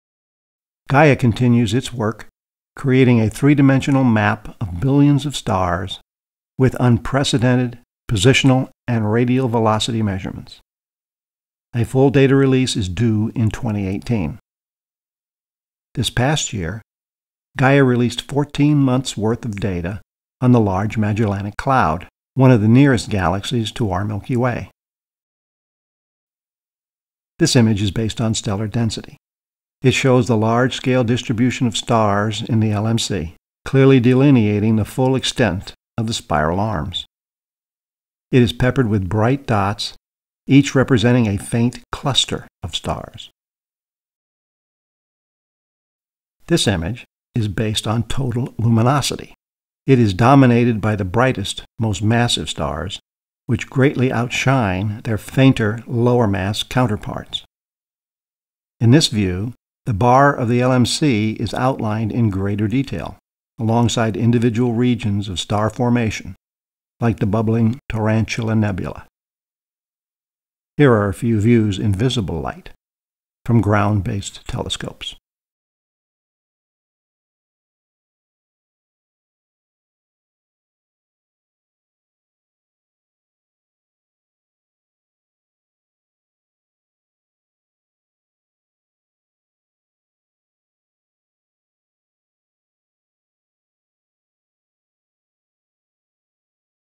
Classroom Aid - Gaia's Large Magellanic Cloud
This is a segment of the "How Far Away Is It" 2017 review.
2017 was another very interesting year with a number of firsts. We detected our first asteroid from another star; a Type 1a Supernova companion survivor; a star collapse into a black hole without a supernova; a gravitationally lensed Type 1a Supernova; a non-elliptical early dead galaxy; colliding neutron stars; and visual confirmation of a gravitational wave.